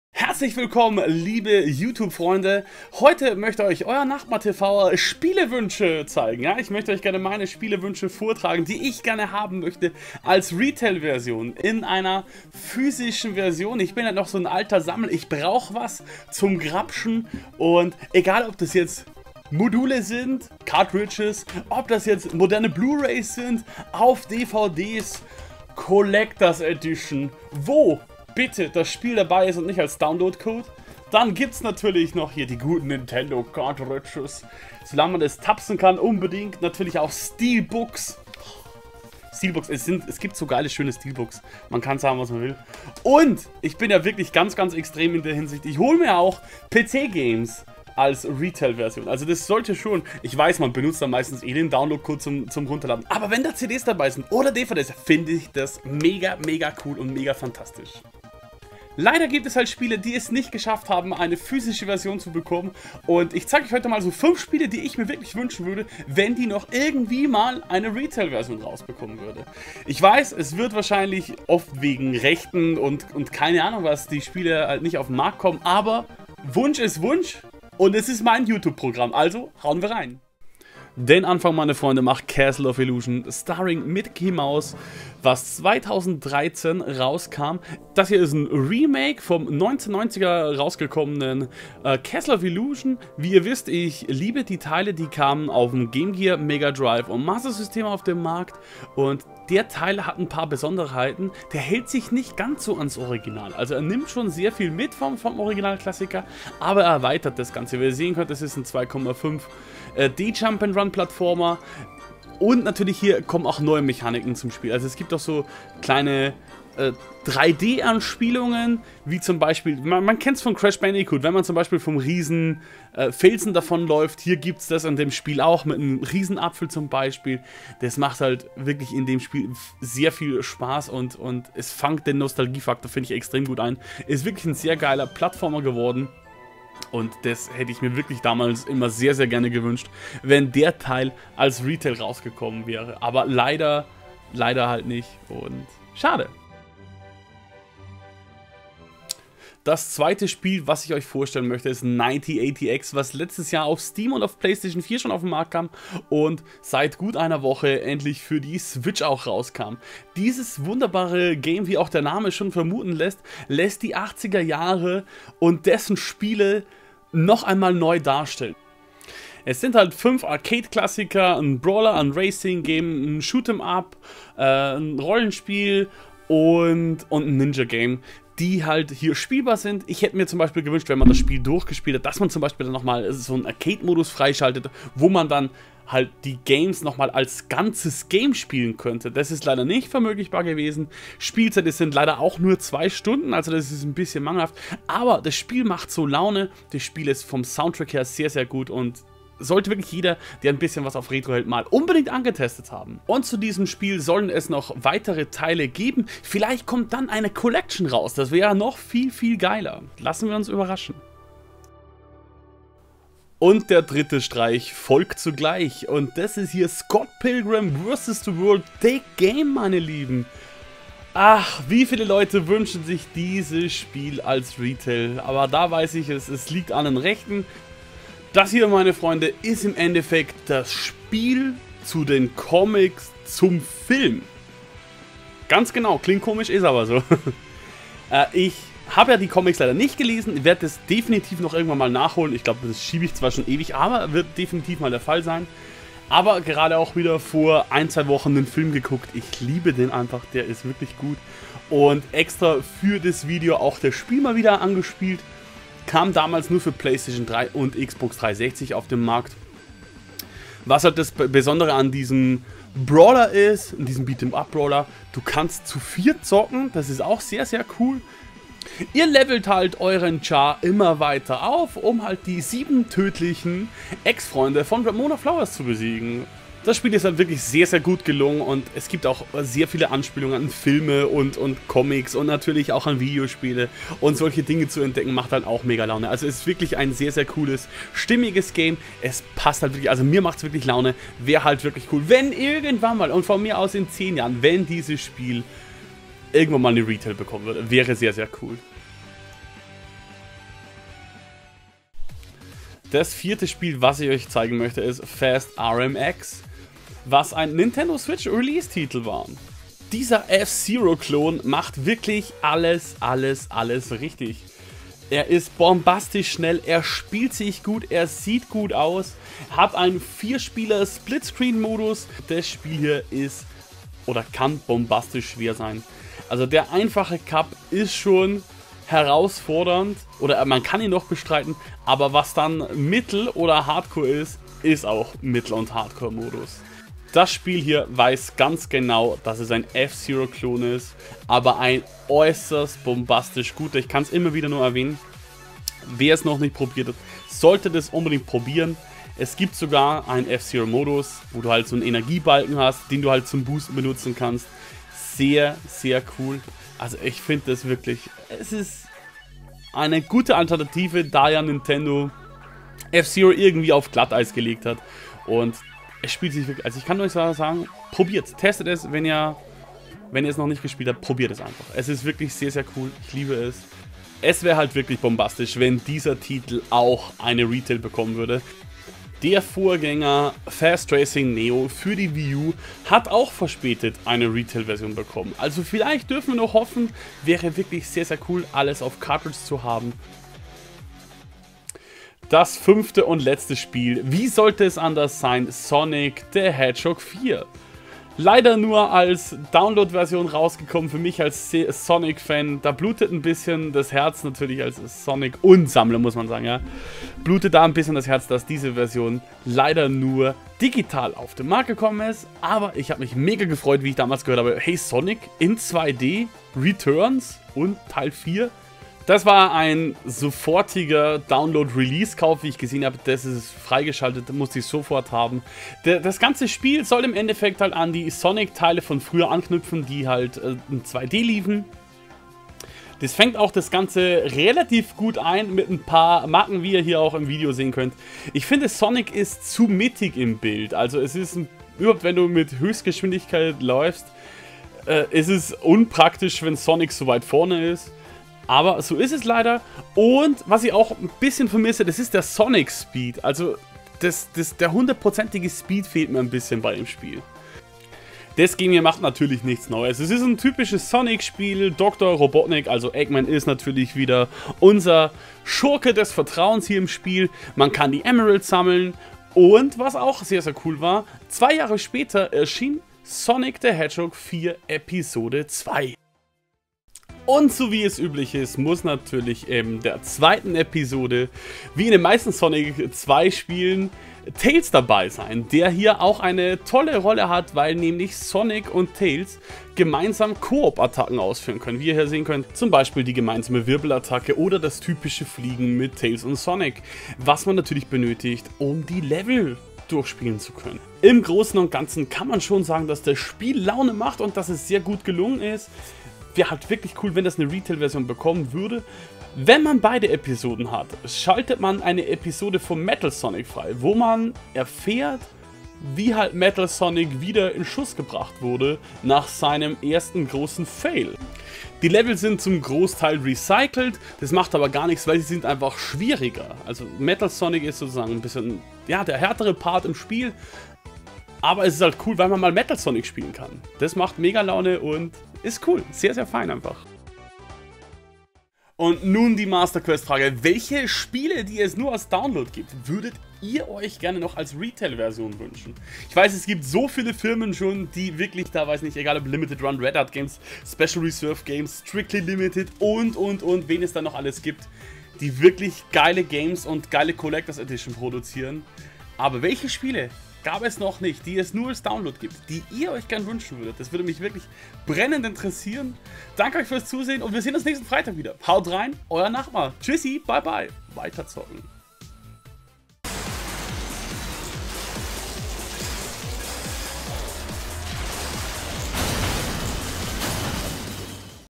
5 SPIELE WÜNSCHE!
Es gibt Games die bekommen leider keine Retail Fassung und da hab ich mir gedacht ich zeig euch mal 5 Spiele die ich mir gerne als physische Version wünschen würde!

Die Sorgen als Spiele Sammler:
"Bekommt das neue GAME eine RETAIL VERSION???"